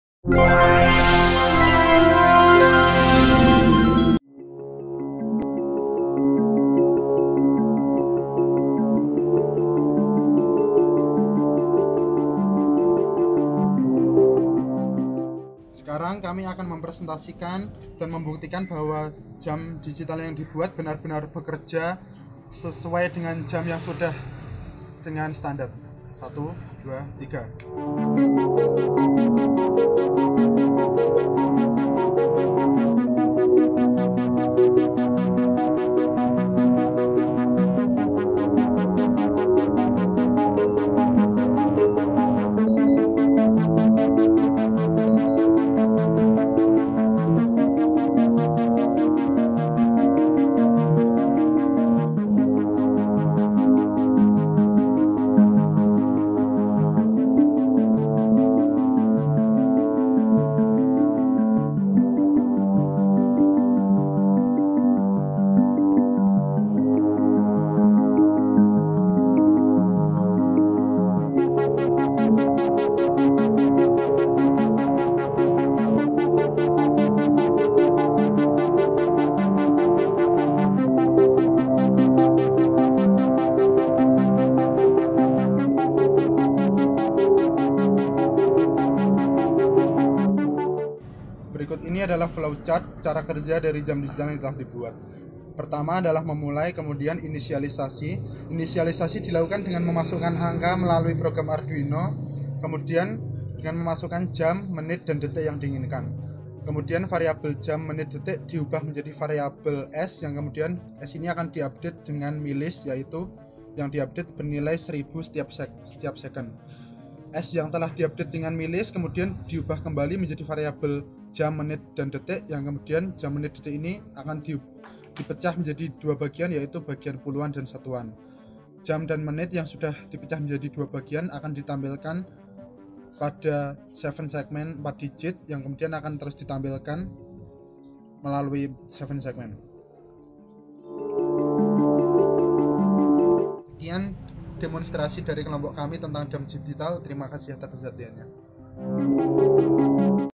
Jam Digital dengan Delay Sederhana
Video ini diunggah untuk memenuhi Tugas Besar I Mata Kuliah EL3214 Sistem Mikroprosesor Semester II-2017/2018 Kelas 03

Implementasi Jam Digital dengan delay sederhana menggunakan ATmega328 (Arduino Uno)

Anggota Kelompok :
1. Tri Agung Susilo - 13214037
2. Tripamungkas Kartika Adi - 13214094
3. Bondan Ari Rahmawan - 13214104